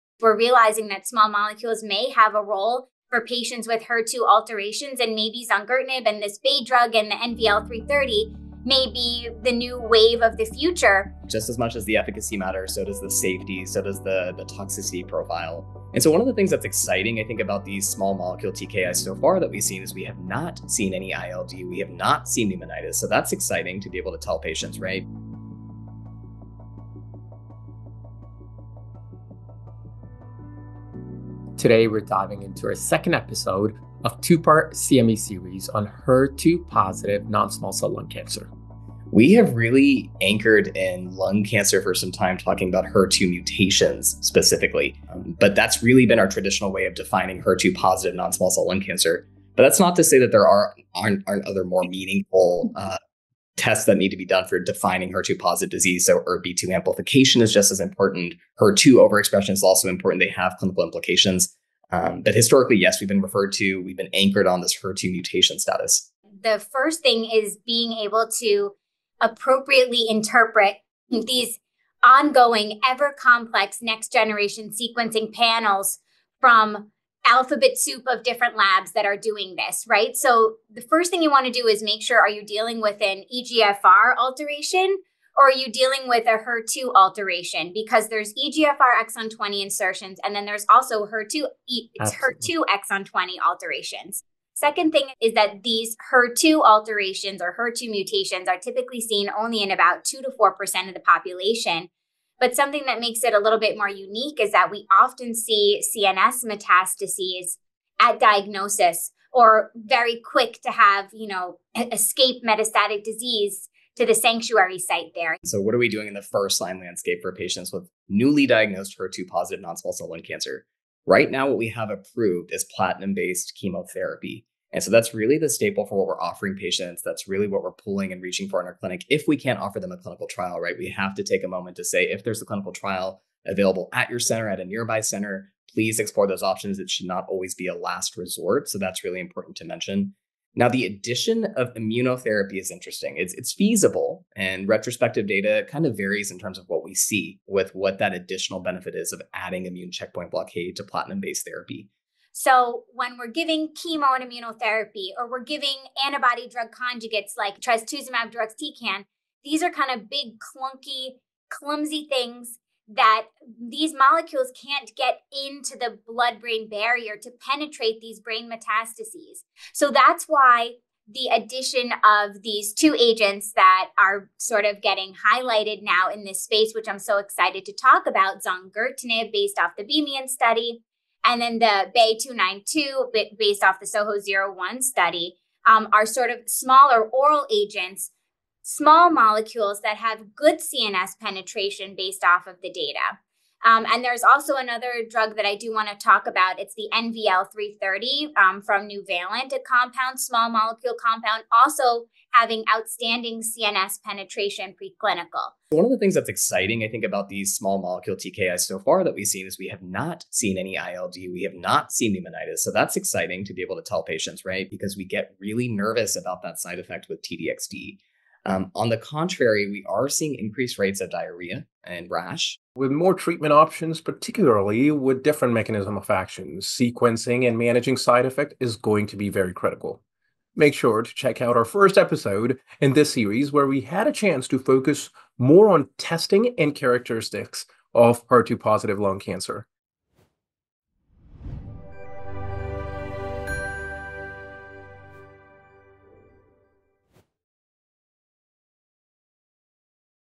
Highlights - The HER2 Diagnostic and Treatment Landscape in NSCLC
This is the second episode of a two-part series on the HER2 diagnostic and treatment landscape in non-small cell lung cancer (NSCLC), hosted by the Oncology Brothers, Drs Rohit and Rahul Gosain.

In this episode, Dr Isabel Preeshagul and Dr Eric Singhi provide the benefit of their experience when discussing how to approach different treatment scenarios in HER2-mutant NSCLC.

The conversation unfolds to cover:
• Ways to distinguish HER2 alterations from other alterations on biomarker reports
• The latest efficacy and safety data of currently approved and emerging treatments for HER2-altered NSCLC
• The potential CNS activity of these treatments in patients with HER2-mutated NSCLC
• How the treatment pathway may look in the near future

Clinical takeaways
• In NSCLC, HER2-positivity includes mutations, amplifications and overexpression. It's important to distinguish HER2 alterations from EGFR mutations, particularly exon 20 insertions, when interpreting next-generation sequencing (NGS) results
• Trastuzumab Deruxtecan (T-DXd) is currently the only approved targeted agent for HER2-altered NSCLC in the 2nd-line setting. It shows promising efficacy, especially in HER2-mutant cases, but has limited brain penetration and is associated with notable side effects, including pneumonitis, which requires close monitoring
• Emerging TKIs, such as zongertinib, BAY 2927088 (sevabertinib), and NVL-330, target HER2-mutations and have shown high response rates and CNS activity in early studies, without ILD/pneumonitis. These treatments come with unique side effects like diarrhoea and rash, which can be managed with supportive care
• CNS metastases are common, with up to 30% of HER2-altered NSCLC patients presenting with or quickly developing CNS metastases. Current large molecule therapies (like T-DXd) have limited brain penetration, making small-molecule TKIs, like zongertinib, BAY 2927088 (sevabertinib), and NVL-330, promising for their potential CNS activity
• Current standard 1st-line care for HER2-mutant NSCLC remains platinum-based chemotherapy ± immunotherapy. Targeted agents (like T-DXd) are generally reserved for 2nd-line use, but ongoing trials are evaluating the move toward frontline therapy